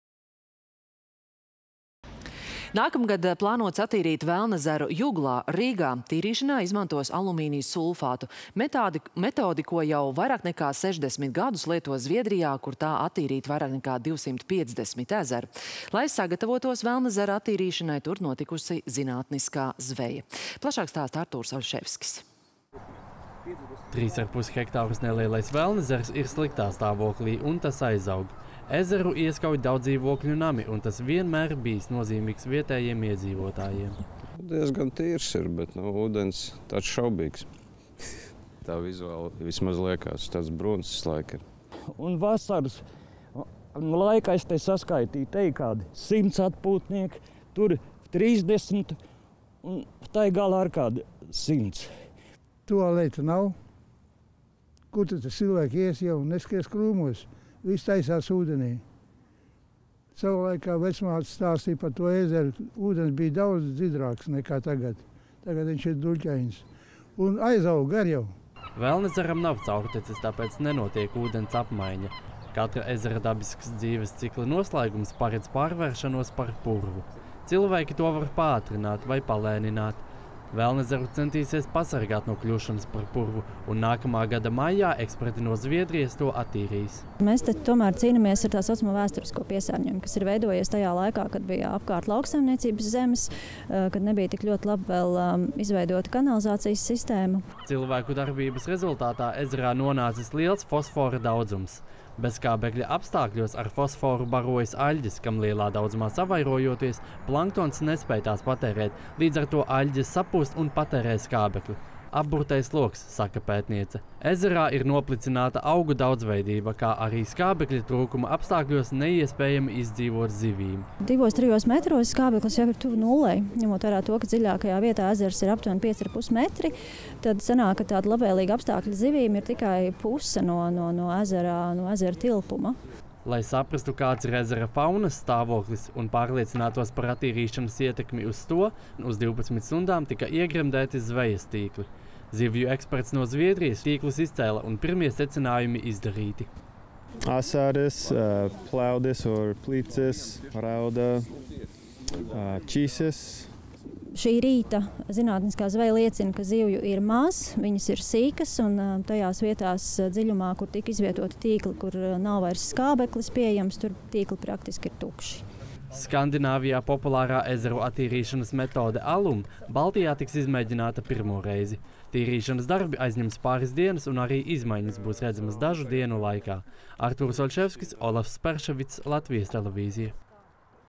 Juglā attīrīšanai gatavo Velnezeru
Nākamgad plānots attīrīt Velnezeru Juglā, Rīgā. Tīrīšanā izmantos alumīnija sulfātu — metodi, ar ko jau vairāk nekā 60 gadus lieto Zviedrijā, kur tā attīrīti vairāk nekā 250 ezeru. Lai sagatavotos Velnezera attīrīšanai, tur notikusi zinātniskā zveja. Plašāk stāsta Artūrs Oļševskis.